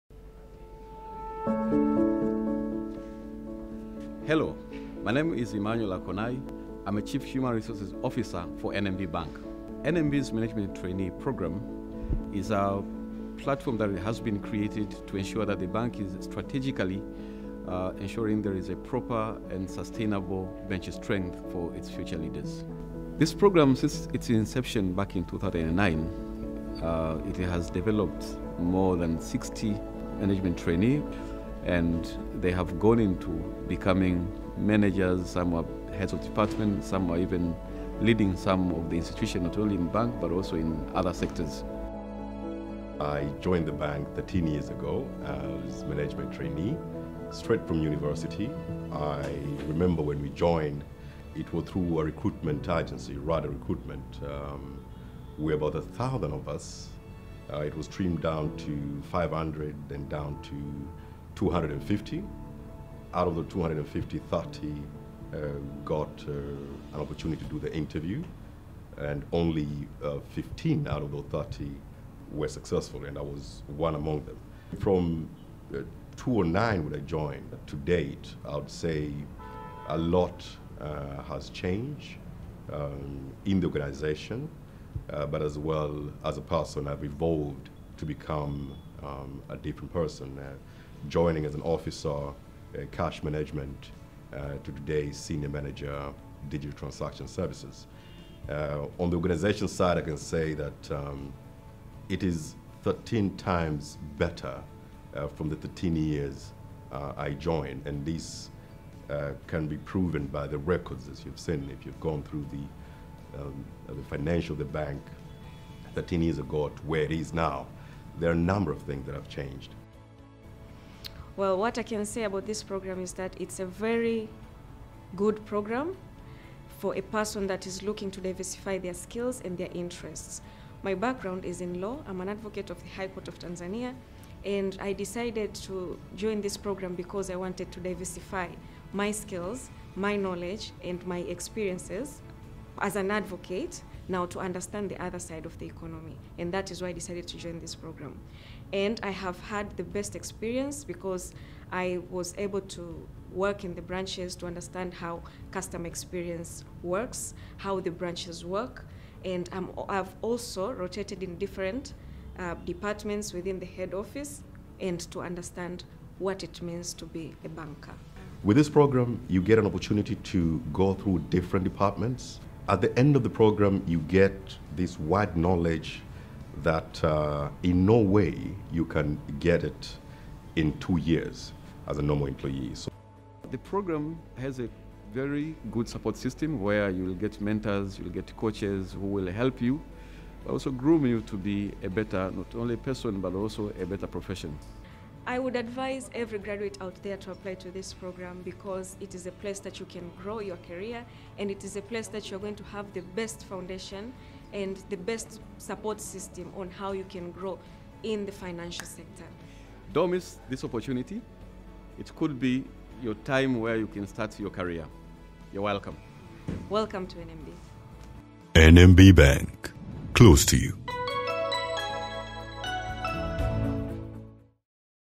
NMB BANK MANAGEMENT TRAINEE PROGRAM
Today, we are incredibly proud and honored that our CEO Ruth Zaipuna has been awarded the prestigious “African Business Leadership Commendation Award” for 2022.

Ms. Zaipuna was awarded the continent’s top business management accolade by the African Leadership Magazine during the just-concluded Africa Summit and African Business Leadership Awards (ABLA) in recognition of her efforts in making NMB Bank impact positively on Tanzanians’ lives and its socio-economic development contribution in the country.

Published by the African Leadership Organization Limited, the influential UK-based magazine principally seeks to celebrate African achievements and excellence by primarily focusing on showcasing the best of Africa to a global audience.

A huge thank you to our customers, shareholders for your ongoing support, this is just the beginning, onward and upward.